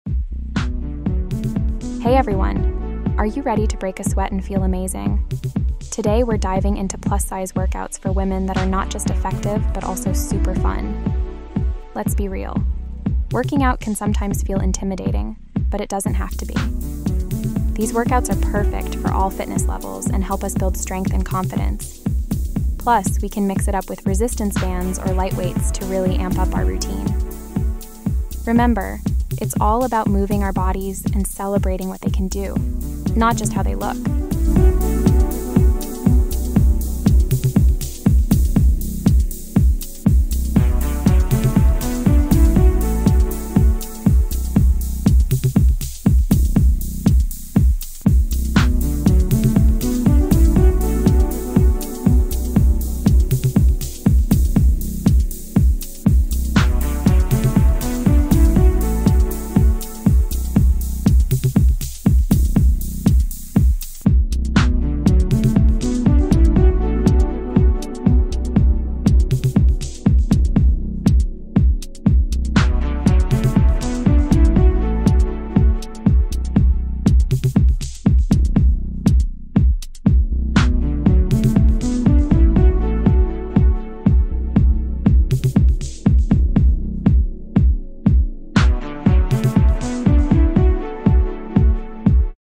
Why Plus Size Workouts Are So Effective!
Discover empowering plus size workouts designed specifically for women! This video showcases a variety of exercises that cater to all fitness levels, ensuring everyone can participate and feel confident.

We’ll explore effective routines that promote strength, flexibility, and overall well-being. Each workout is tailored to inspire and motivate us on our fitness journey while celebrating body positivity. Join us as we break down barriers and redefine what it means to be fit at any size!